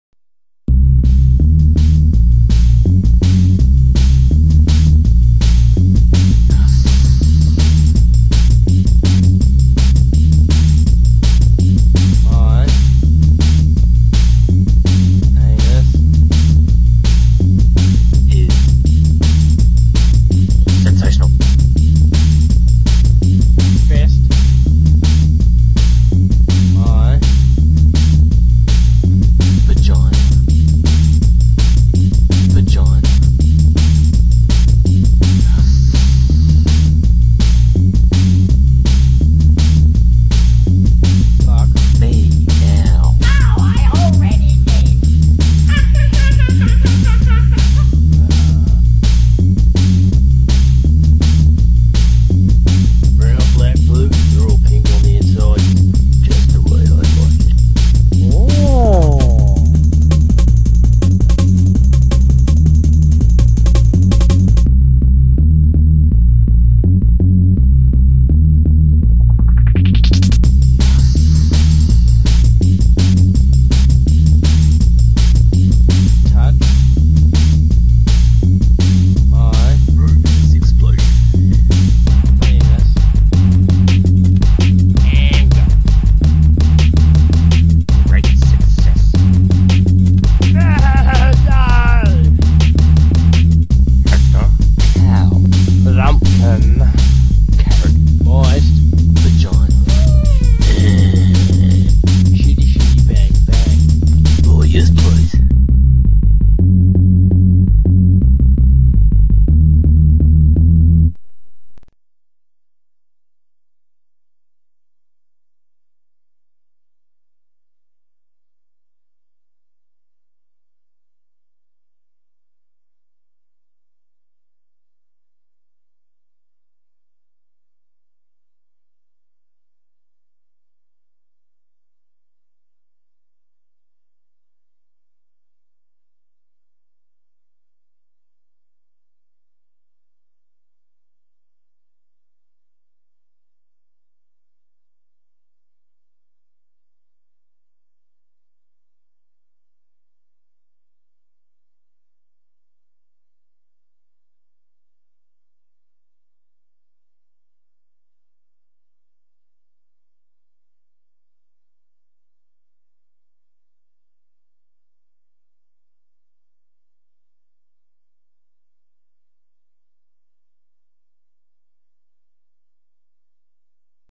Fake boner in public
This is a vid me n my friend made about 4 years ago. I lost the original audio so i added a song that i made.
filf out. Also, the magazine i was reading is a magazine dedicated to pokemon.